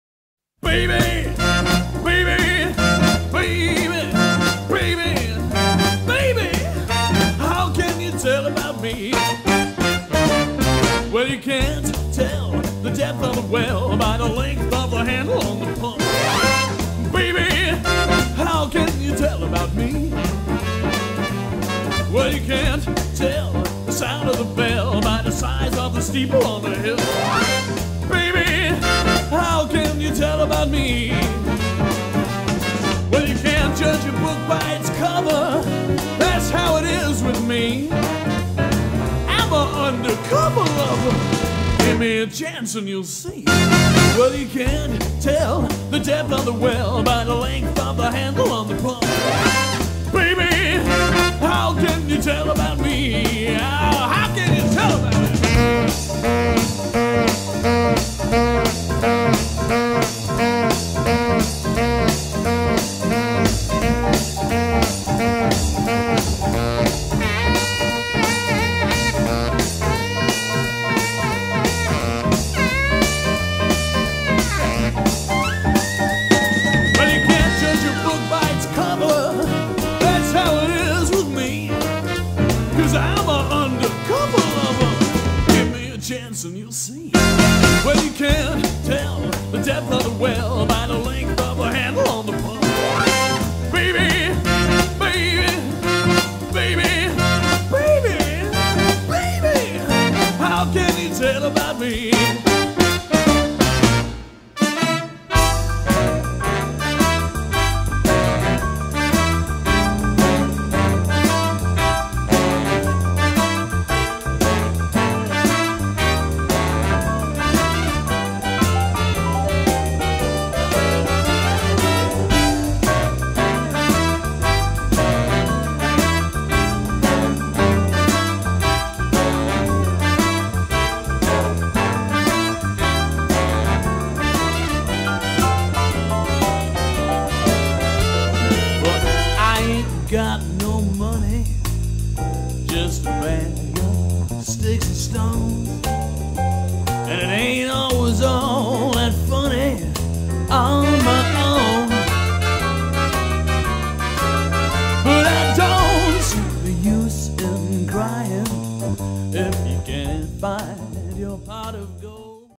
Abseiling Sax
A charity abseil, raising money for kidney research UK, featuring Andy Williamson, saxophonist and kidney dialysis patient.